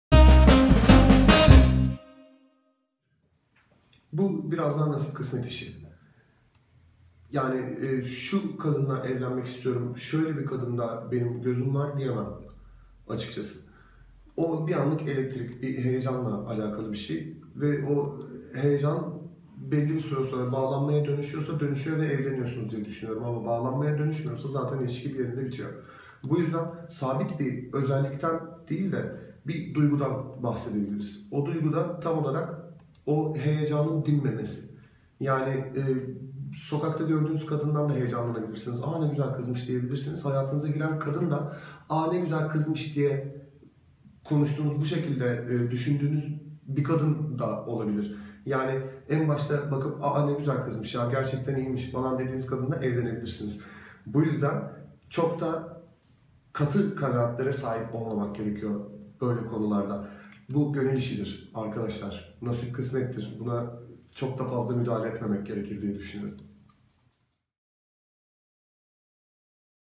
Nasıl bir kadınla evlenmek istersin?
Alican Albayrak KizlarSoruyor'da cevaplıyor